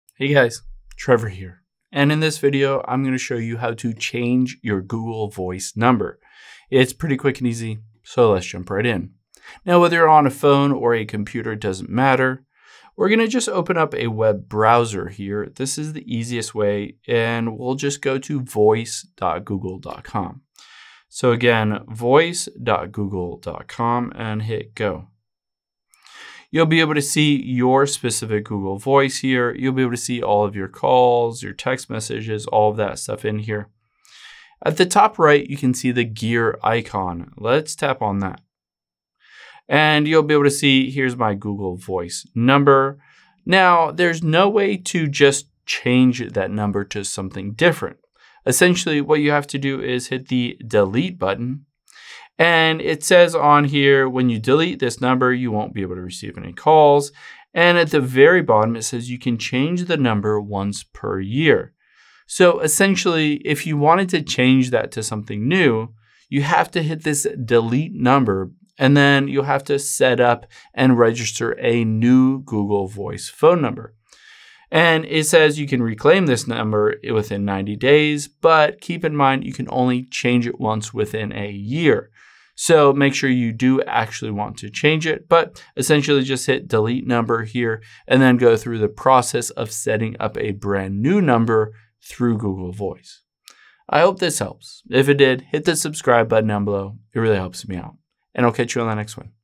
How To Change Google Voice Number
Here's how you can change your Google Voice number if you want to reset it or change the area code for your number.